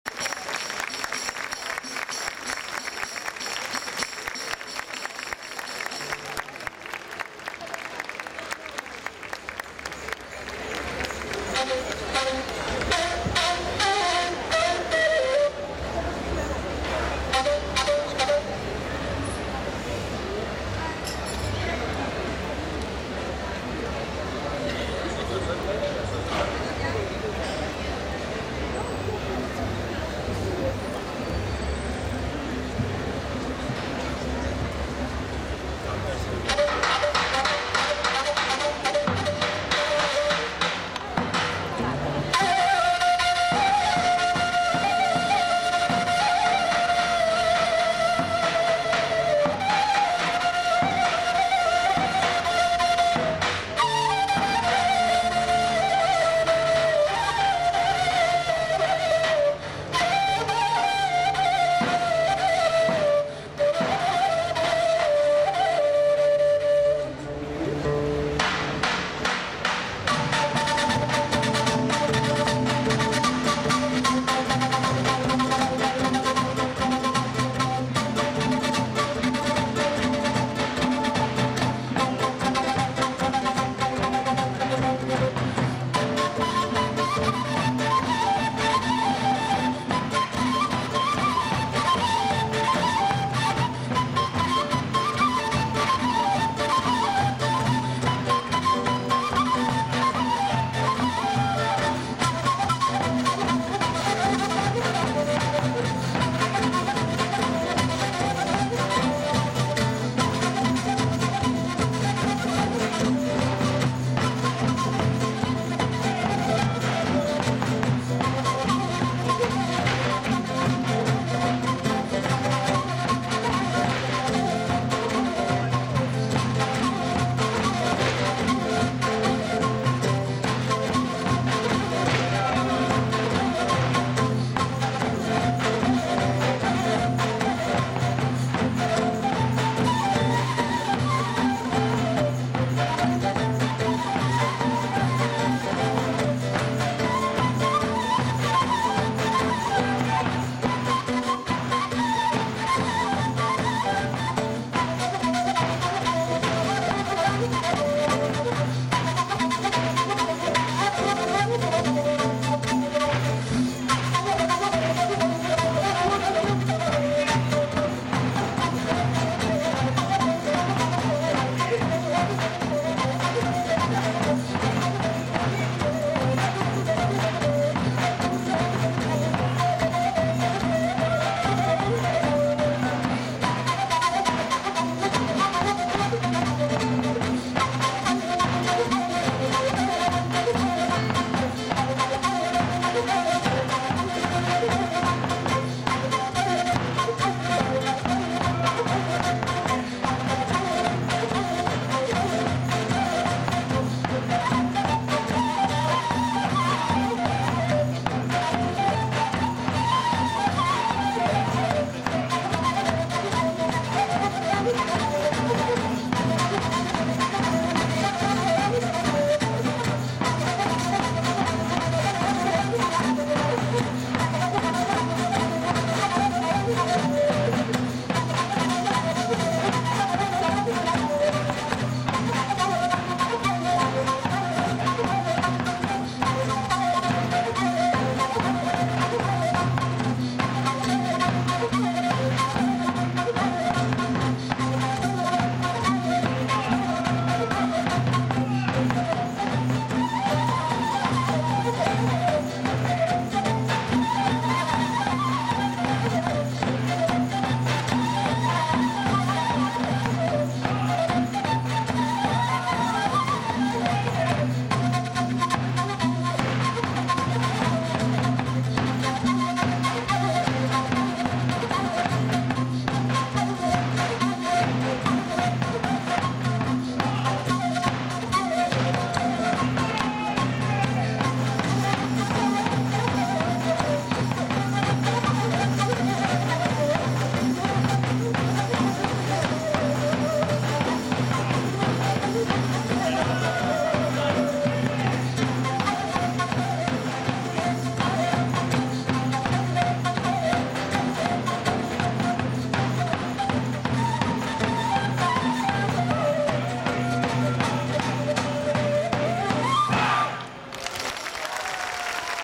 Turkish traditional folk dance: Karadeniz
Turkish traditional folk dance by Izmir Folklor Artirma Kurumru, from Izmir, Turkey. Recorded in A Coruña, Galicia, Spain, on 20/08/2011.

Danzas tradicionais folclóricas turcas. Izmir Folklor Artirma Kurumru, de Izmir, Turquía. Gravado o 20/08/2011 na Coruña durante o XXV Festival Internacional de Folclore Cidade da Coruña.